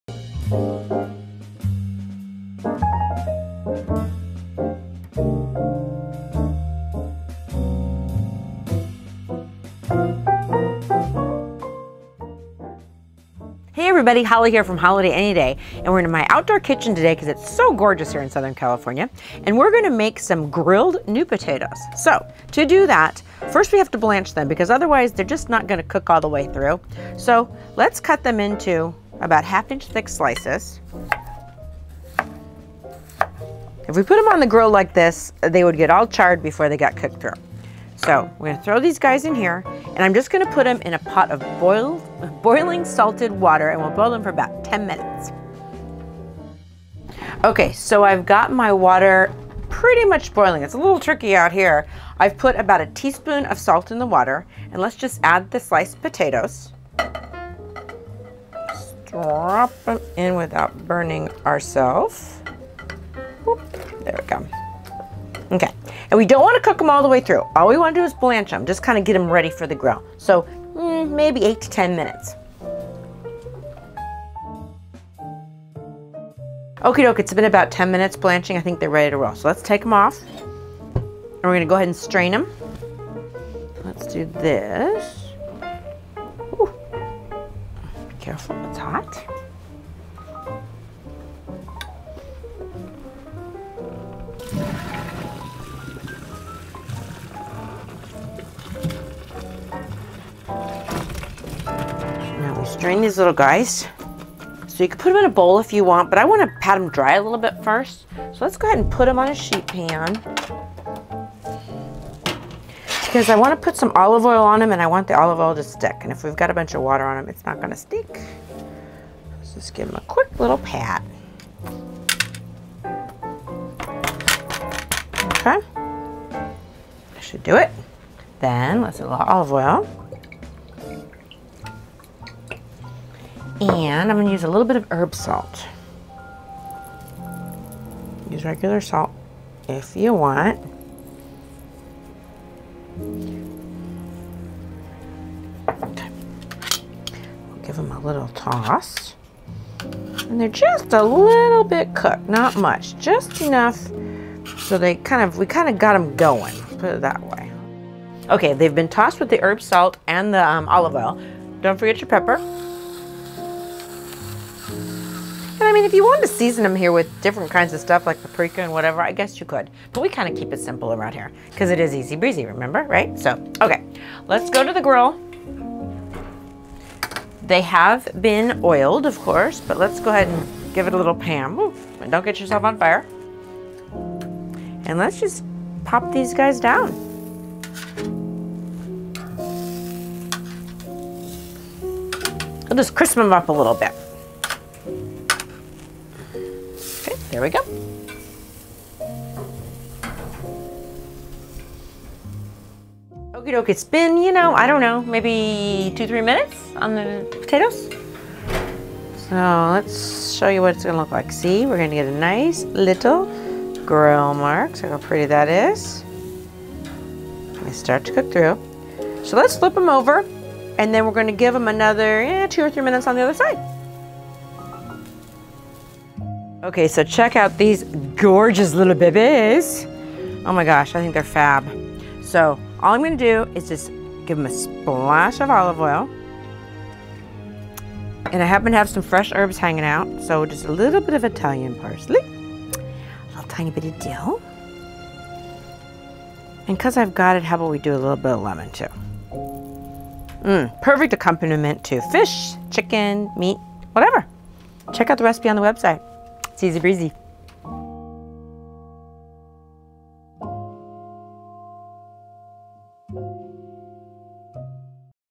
HolliDay AnyDay: Easy Breezy Kitchen - Grilled Herbed Red Potatoes
Grilled Herbed Red Potatoes
serves two

4 red potatoes, sliced about 1/2 inch thick
1 teaspoon salt
assorted chopped fresh herbs such as italian parsley, thyme, dill and chives
lemon
olive oil

Preheat your grill to medium high heat.
Fill a medium saucepan with water, add salt. Bring to a boil. Add potatoes and reduce to a low
boil, boil about 5 minutes until softened slightly.
Remove potatoes from saucepan and lay on paper towels, blot to dry.
Toss with olive oil.
Coat grill with cooking spray. Place potatoes on grill and grill about 3 minutes per side until
cooked through and the potatoes have nice grill marks.
Drizzle with olive oil, fresh herbs and a spritz of lemon juice.